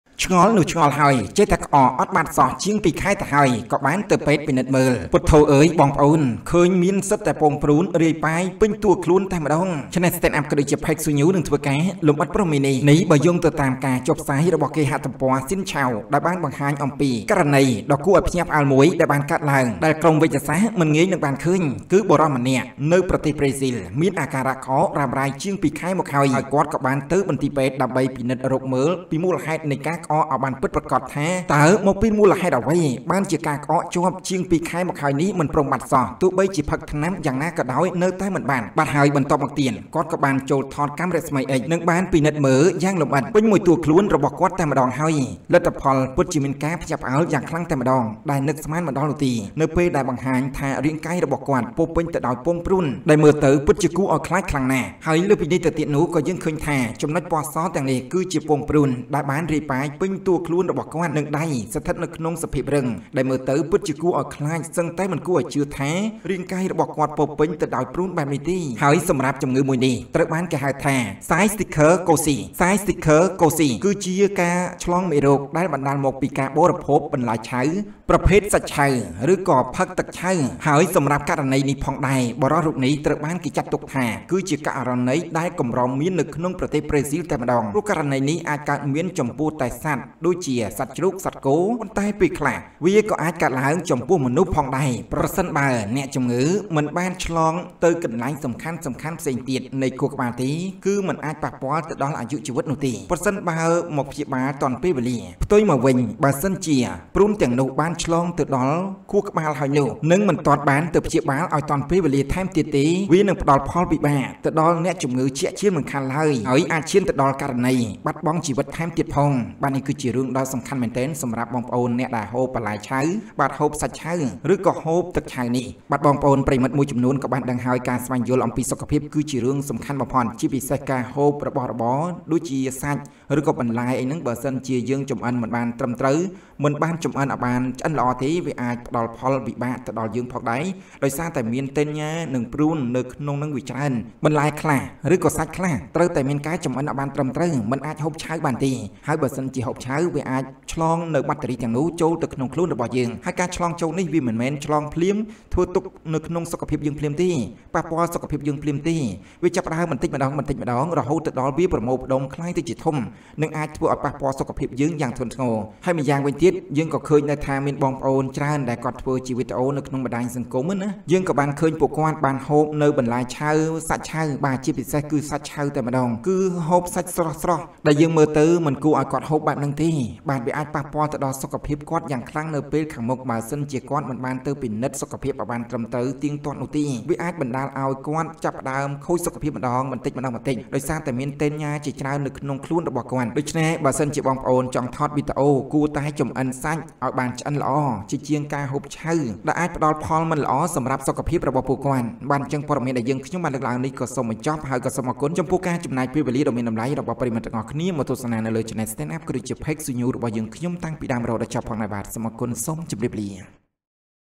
​ក្តៅៗ បុរសម្នាក់ក្អកអត់បាត់ជាង ២ ខែ ទៅពេទ្យពិនិត្យមើល លោកអើយឃើញមាន​ពងព្រូនរាយប៉ាយតួពេញខ្លួន
​ក្តៅៗ បុរសម្នាក់ក្អកអត់បាត់ជាង ២ ខែ ទៅពេទ្យពិនិត្យមើល លោកអើយឃើញមាន​ពងព្រូនរាយប៉ាយតួពេញខ្លួន, Stand Up